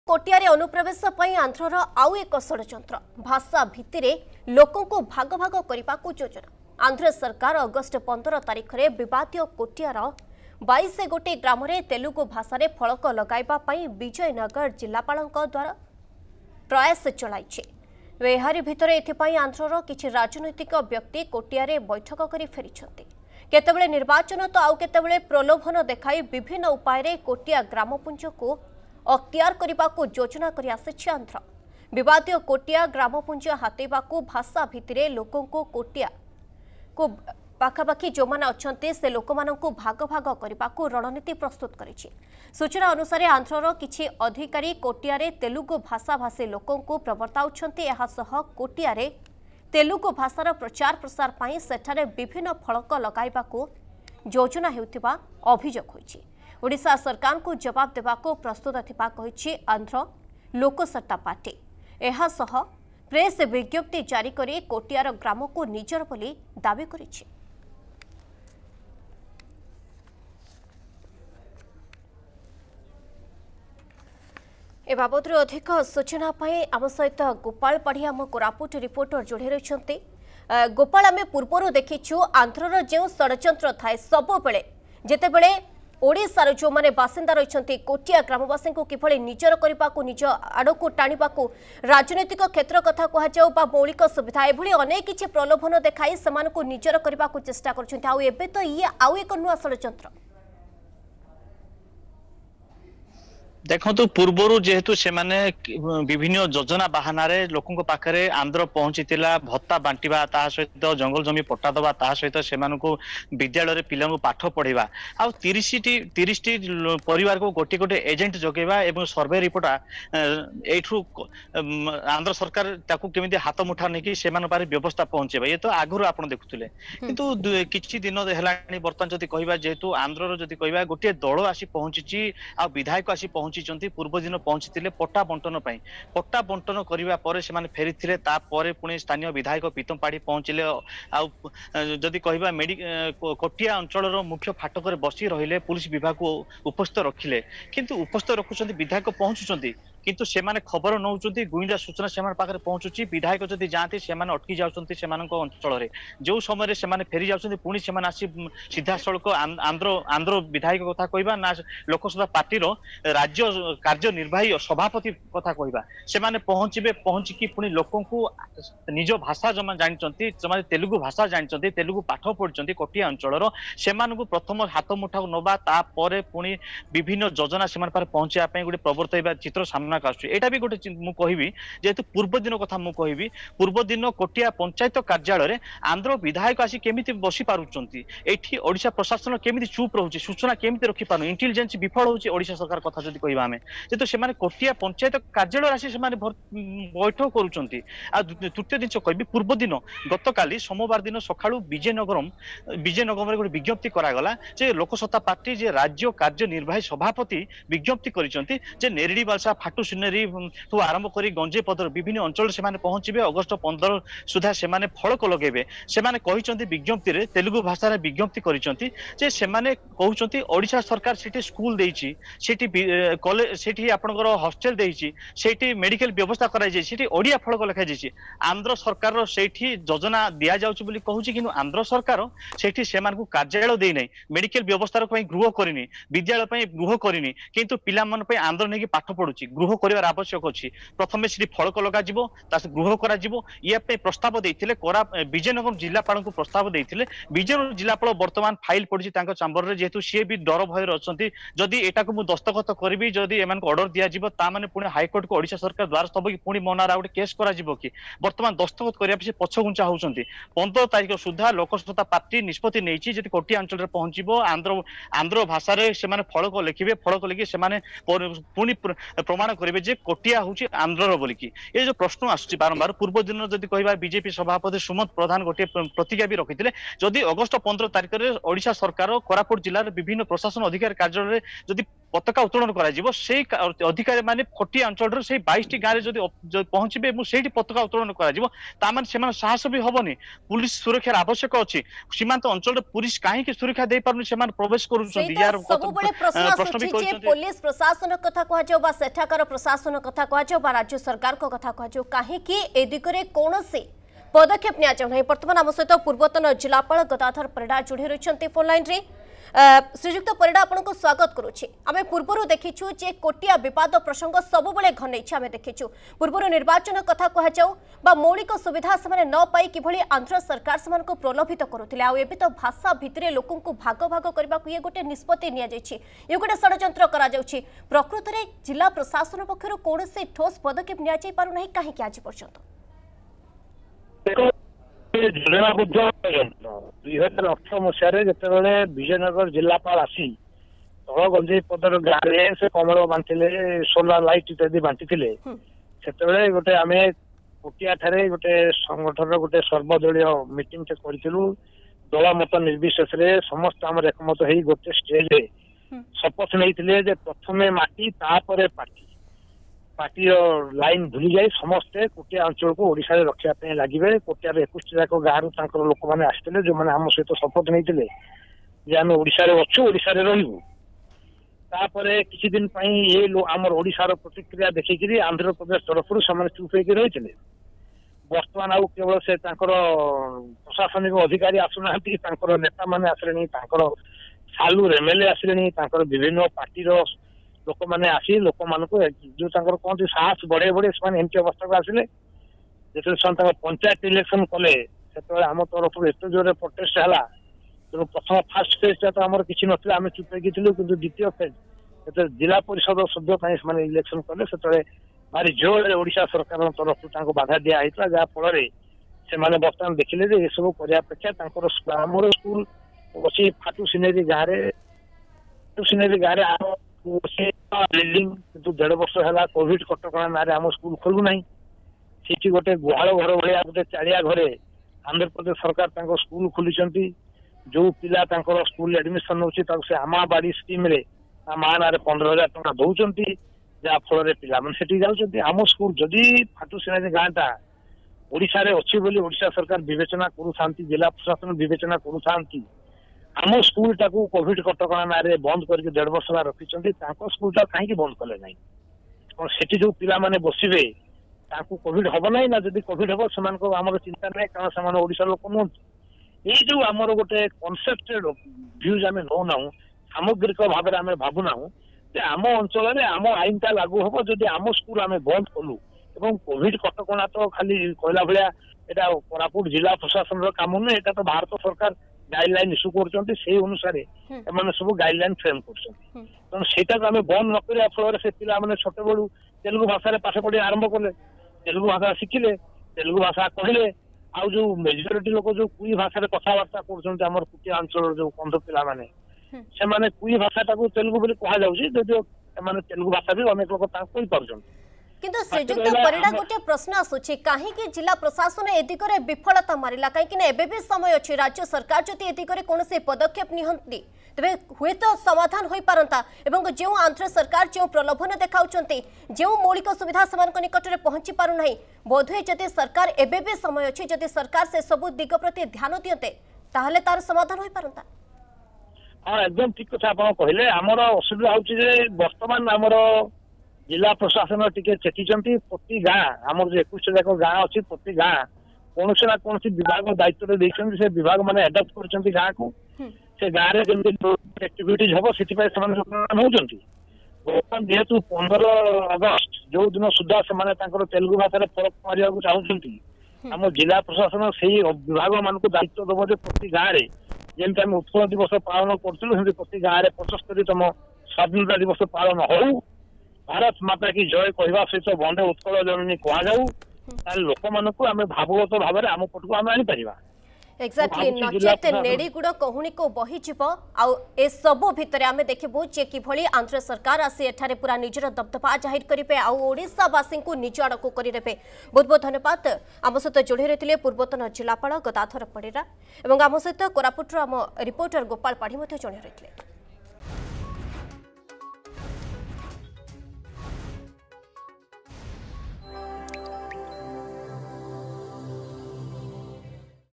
କୋଟିଆରେ ଅନୁପ୍ରବେଶ ପାଇଁ ଆନ୍ଧ୍ରର ଆଉ ଏକ ଷଡ଼ଯନ୍ତ୍ର
ଭାଷା ଭିତ୍ତିରେ ଲୋକଙ୍କୁ ଭାଗ ଭାଗ କରିବାକୁ ଯୋଜନା ।  ଆନ୍ଧ୍ର ସରକାର ଅଗଷ୍ଟ 15 ତାରିଖରେ ବିବାଦୀୟ କୋଟିଆର 22 ଗୋଟି ଗ୍ରାମରେ ତେଲୁଗୁ ଭାଷାରେ ଫଳକ ଲଗାଇବା ପାଇଁ ବିଜୟ ନଗର ଜିଲ୍ଲାପାଳଙ୍କ ଦ୍ଵାରା ପ୍ରୟାସ ଚାଲିଛି । ଏହାଭିତରେ ଏଥିପାଇଁ ଆନ୍ଧ୍ରର କିଛି ରାଜନୈତିକ ବ୍ୟକ୍ତି କୋଟିଆରେ ବୈଠକ କରି ଫେରିଛନ୍ତି